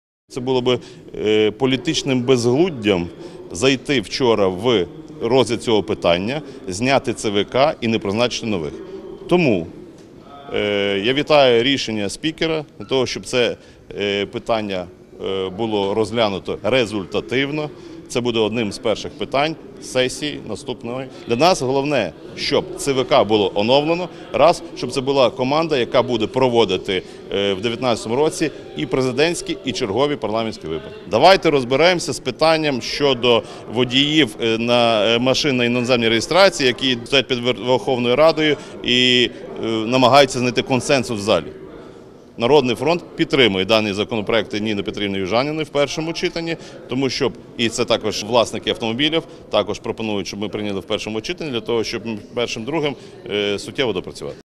Питання ЦВК буде одним із перших наступної сесії парламенту, - Максим Бурбак
"Народний фронт" вітає рішення президентської фракції щодо відкликання однієї кандидатури до складу Центральної виборчої комісії, яким вони розблокувати проблему 14 кандидатів на 13 посад.

Про це сказав керівник фракції "Народний фронт" Максим Бурбак, спілкуючись із журналістами в кулуарах парламенту.

Проте розглядати питання ЦВК в четвер на вечірньому засіданні було безвідповідально,  зважаючи на важливість розгляду законопроекту, яким врегульовується ситуація із блокуванням Верховної Ради водіями авто на іноземній реєстрації.

"Це було б політичним безглуздям зняти членів ЦВК і не проголосувати за нових. Тому "Народний фронт" вітає рішення спікера, щоб це питання було розглянуто результативно. Це буде одним із перших питань наступної сесії",  - повідомив Максим Бурбак .

Він наголосив, що для "Народного фронту" питання оновлення ЦВК є важливим.

"Щоб це була команда, яка буде проводити в 2019 році і президентські, і чергові парламентські вибори ", - наголосив керівник фракції.

Він також підкреслив, що у четвер важливішим питанням було розгляд у першому читанні законопроекту щодо врегулювання ситуації з автомобілями на іноземній реєстрації.

"Давайте розберемось з питанням щодо водіїв машин на іноземній реєстрації, які стоять під Верховною Радою і намагаються знайти консенсус в залі",  - закликав колег Максим Бурбак.

"Народний фронт" підтримує даний законопроект Ніни Петрівни Южаніної в першому читанні. І це також власники автомобілів пропонують, щоб ми прийняли в першому читанні, щоб до другого читання доопрацьовувати",  - наголосив керівник фракції.

Приєднуйся!